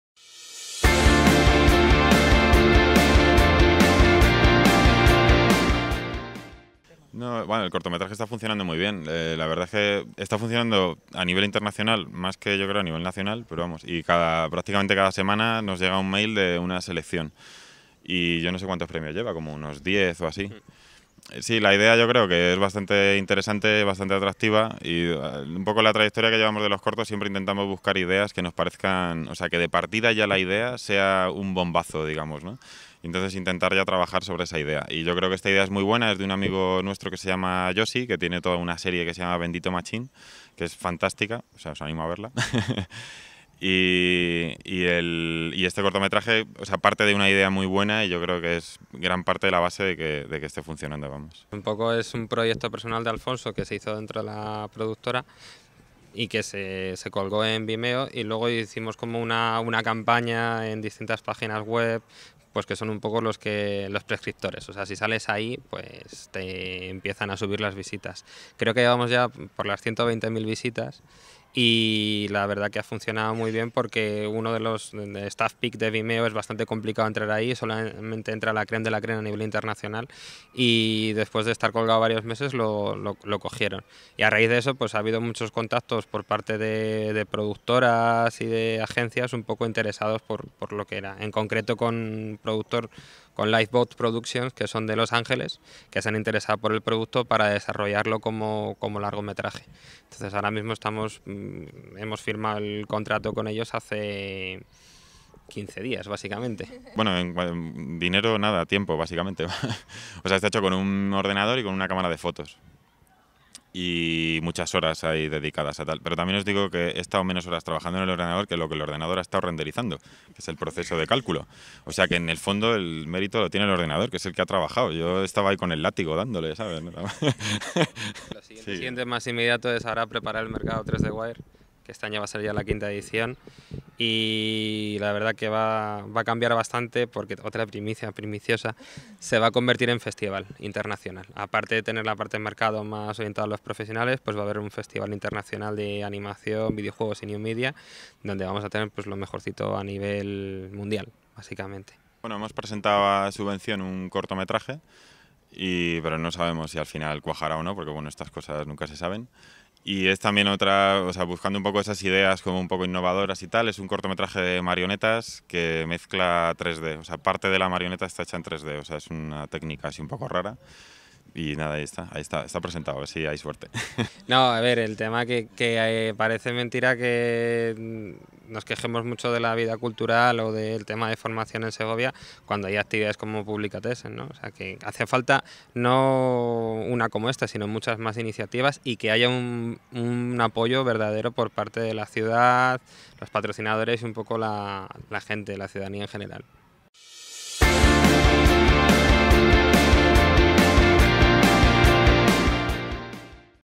Fulgencio y Farias, destapando los últimos proyectos de Paramotion Films
Una productora de Los Angeles está interesada en hacer un largo de la 'Historia de robots' de la empresa segoviana Paramotion Films. Pequeña historia de coste cero que ha sido seleccionada entre los mejores vídeos por la plantilla de la red social Vimeo. Desde entonces, acumula 120.000 visionados y una productora norteamericana ha firmado moverla. Es una de las "primicias primiciosas" que nos contaron José Luis Farias y Alfonso Fulgencio a la salida de la charla que dieron este martes en Publicatessen.